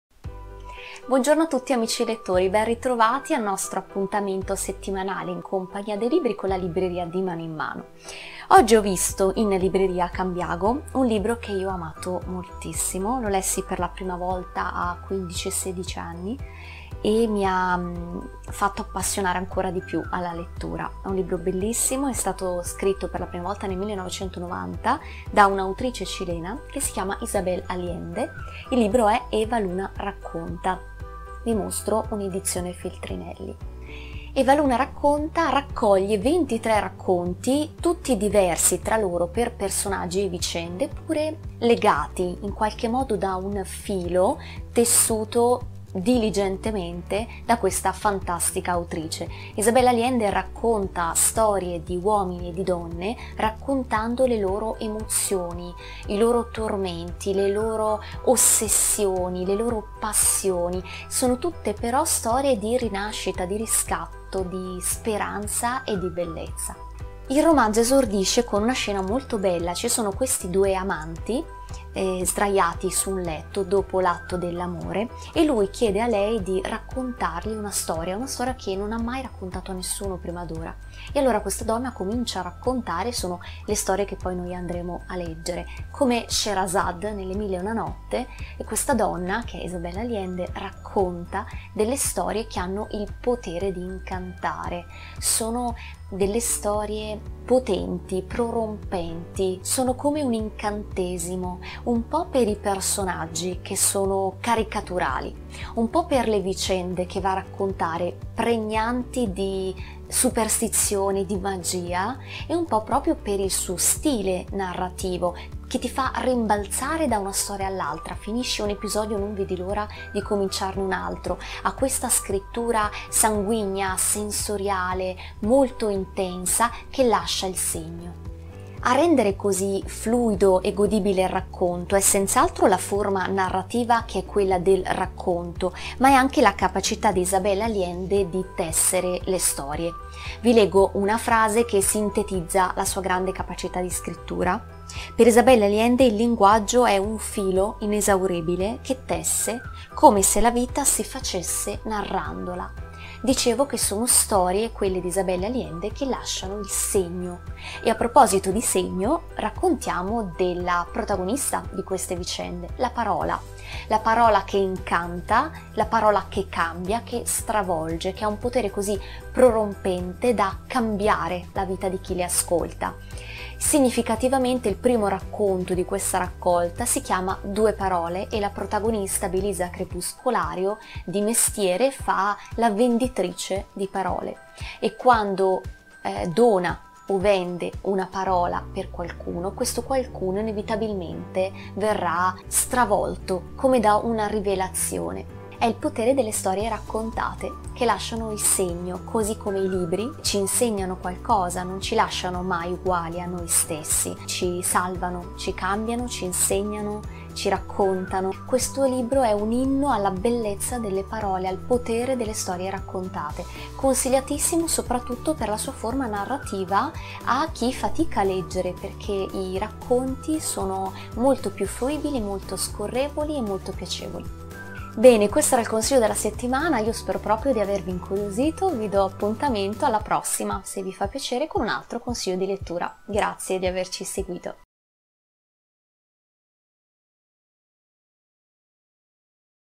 consiglidilettura Eva Luna racconta di ISABEL ALLENDE
In questo video Raffaella ci racconta il romanzo Eva Luna di Isabel Allende